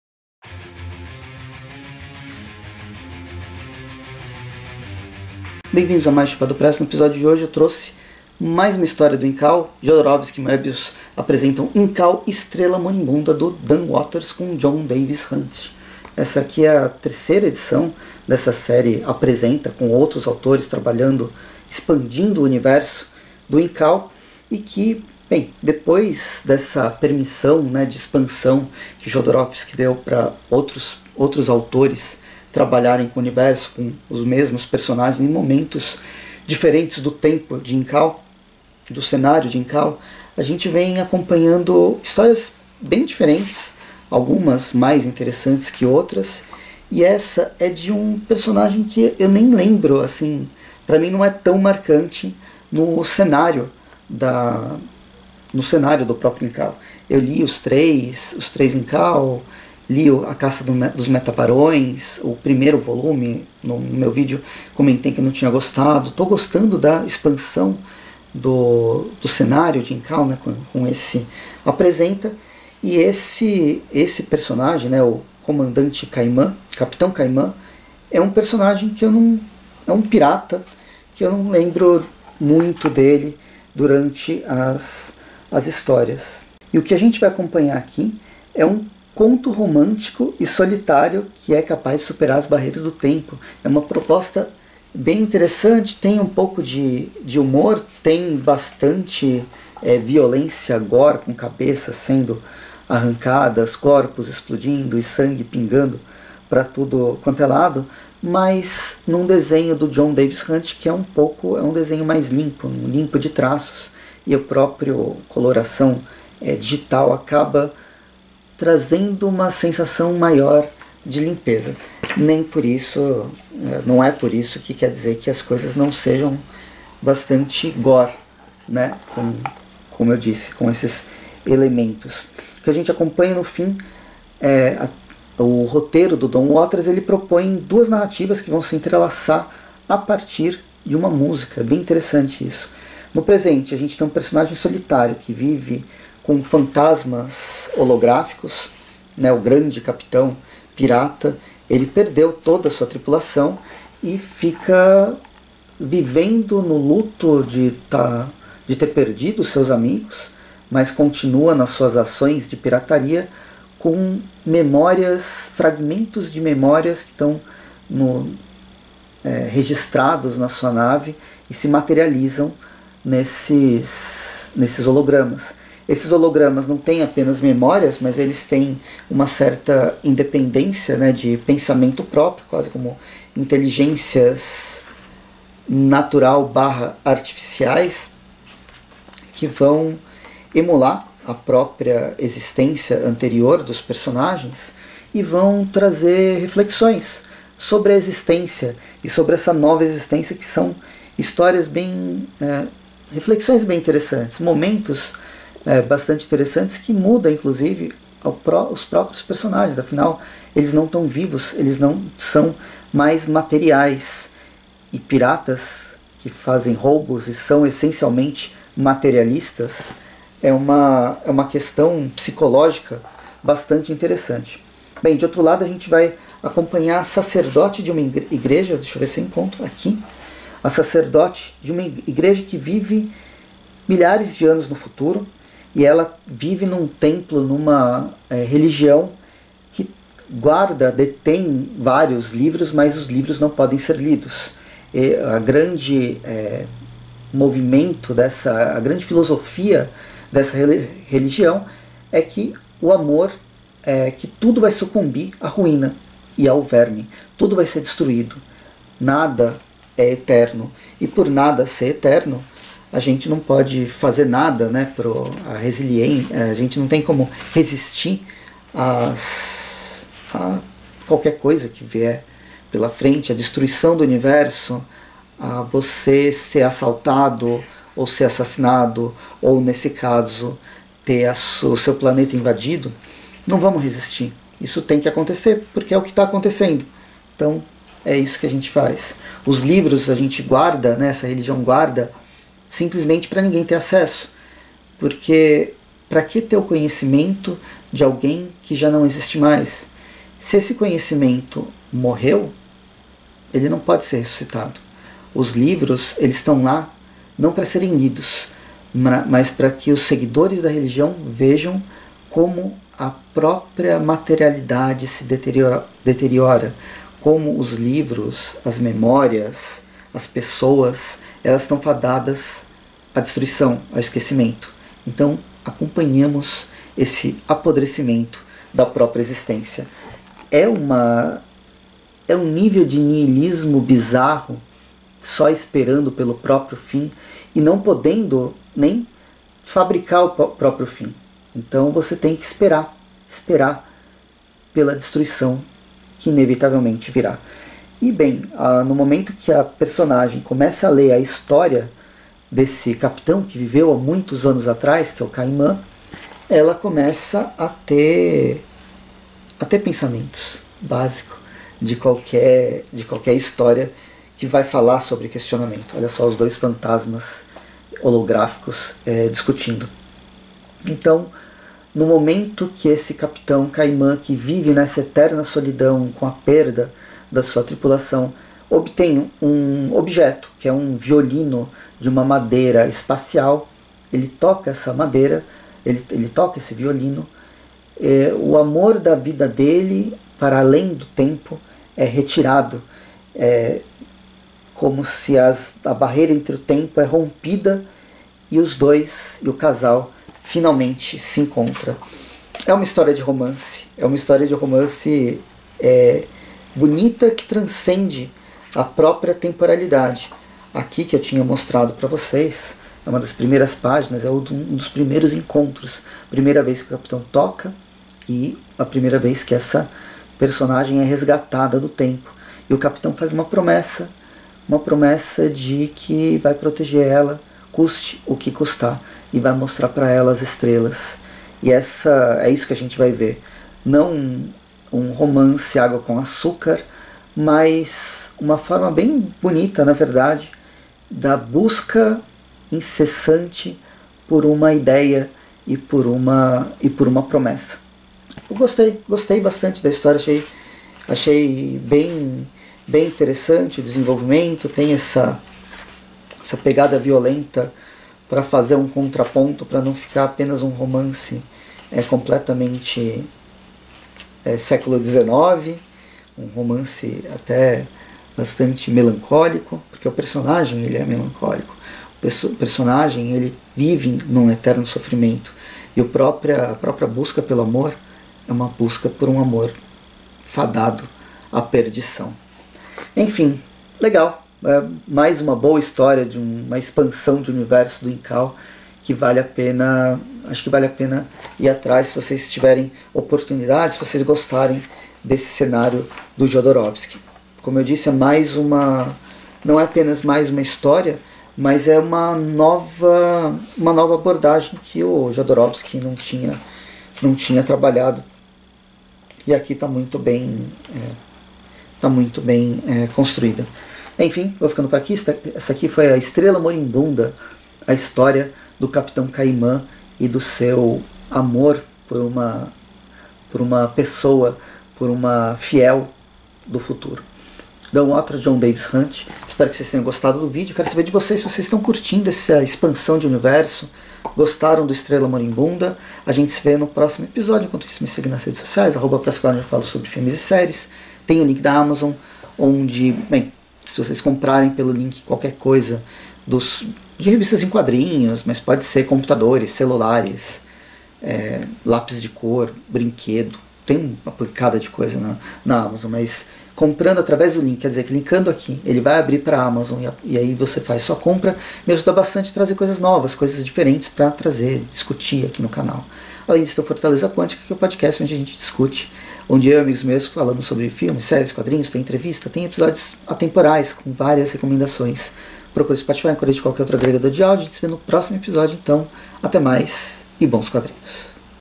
Chapéu do Presto - Estrela Moribunda no universo de Incal
Bem-vindos a mais um Chapéu do Presto, no episódio de hoje apresento mais uma expansão do universo de Incal, a história do capitão Kaimann Estrela Moribunda, por Dan Watters e Jon Davis-Hunt.